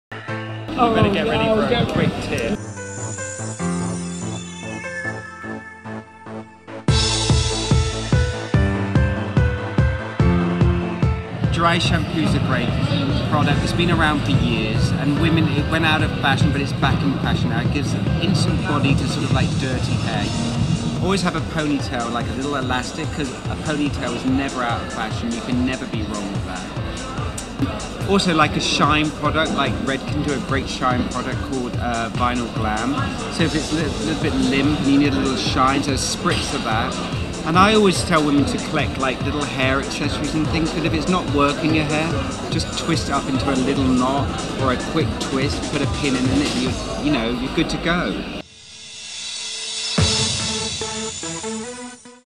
Hair Care On the Go
Stylist to the stars "Guido" dishes on what makes good hair! Hear why Dry Shampoo could be the best secret weapon against frizzy hair and grease -- and what a woman should never leave home without!